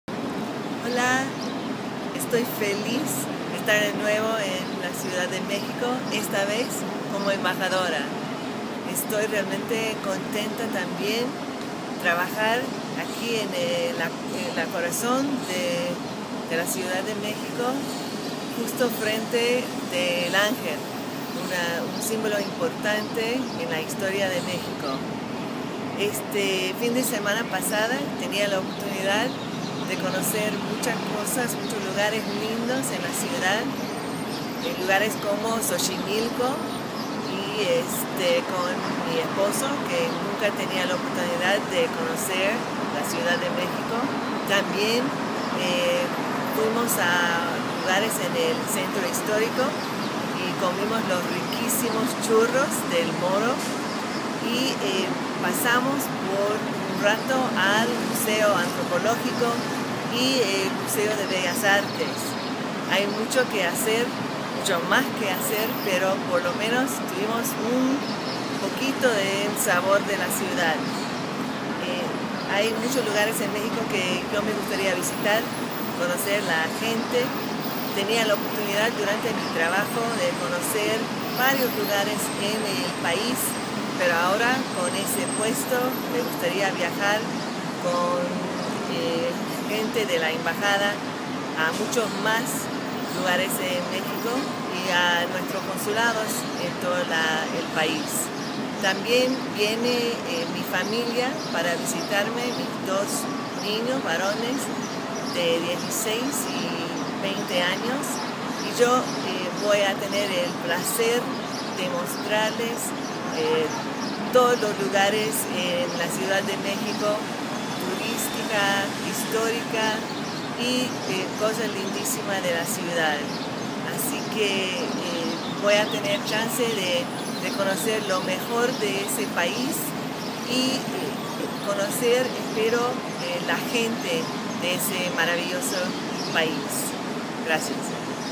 Roberta Jacobson desde Embajada de Estados Unidos en Reforma
‪#‎EmbRoberta‬ - Ya instalada en nuestro edificio de la Embajada de Estados Unidos en Reforma. ‪#‎CDMX‬